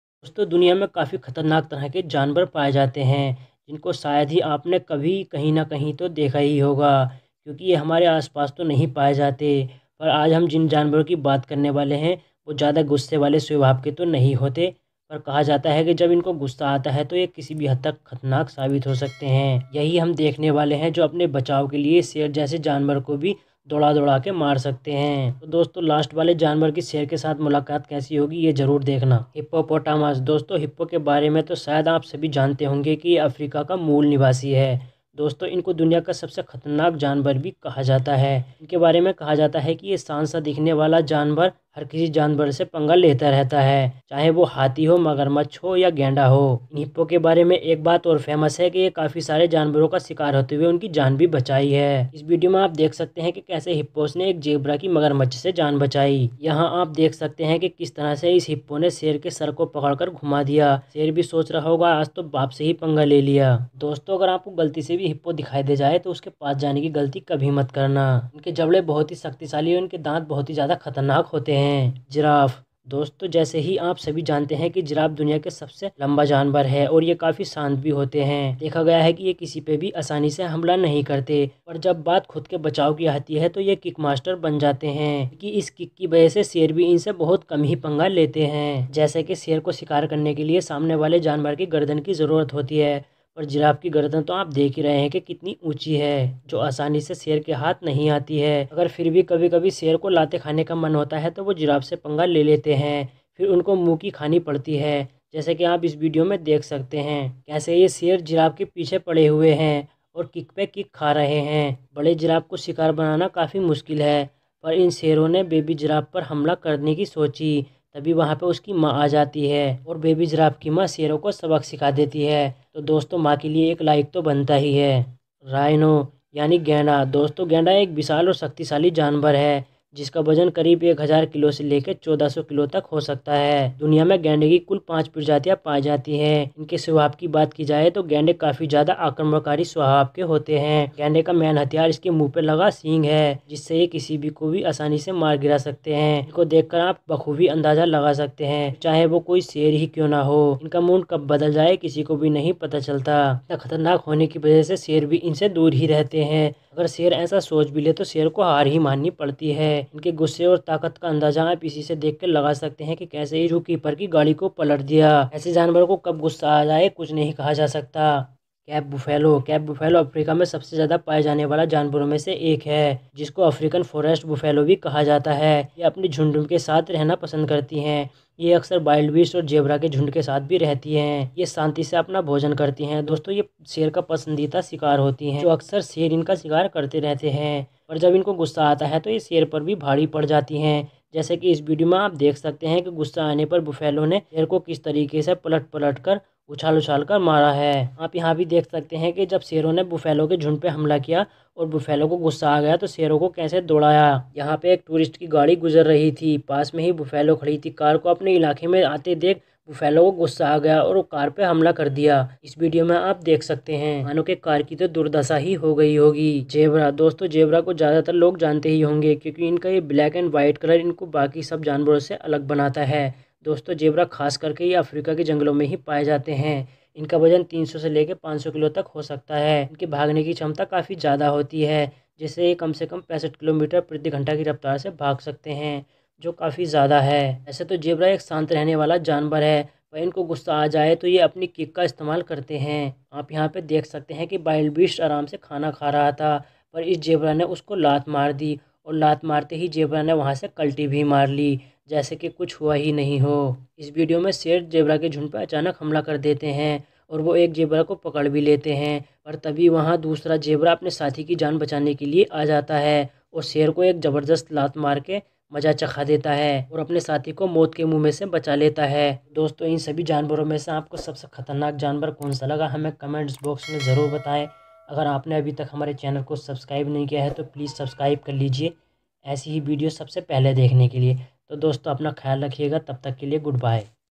जानवरो के हैरान कर देने वाले कारनामे | Most Powerful And Amazing Animals | Amazing Adventures हिन्दी
Dosto is video mein janwaro ke kuch behad he ajeeb O Gareeb Karnamo ko dikhaya gaya hai Jinke bare me Ap soch Bhi Nahi Sakte